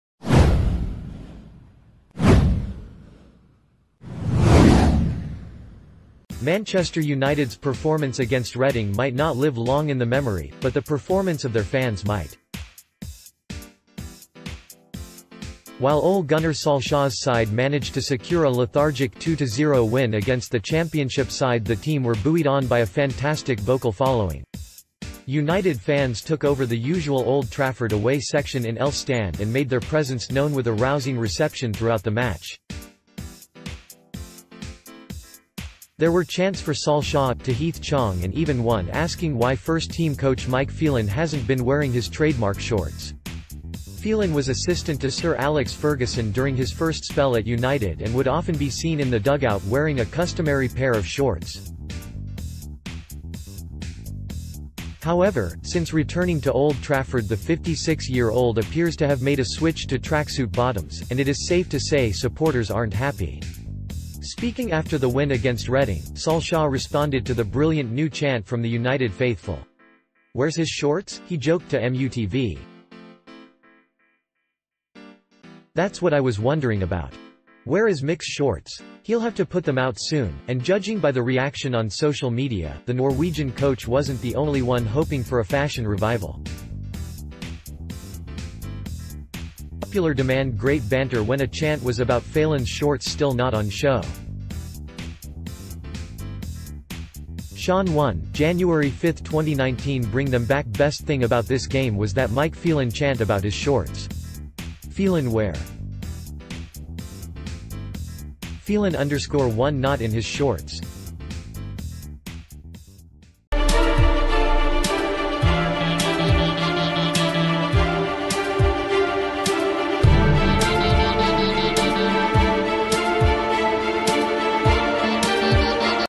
Solskjaer loved Man United fans chant about Phelan's shorts
Manchester United's performance against Reading might not live long in the memory, but the performance of their fans might. While Ole Gunnar Solskjaer's side managed to secure a lethargic 2-0 win against the Championship side the team were buoyed on by a fantastic vocal following. United fans took over the usual Old Trafford away section in L Stand and made their presence known with a rousing reception throughout the match. There were chants for Solskjaer, Tahith Chong and even one asking why fi...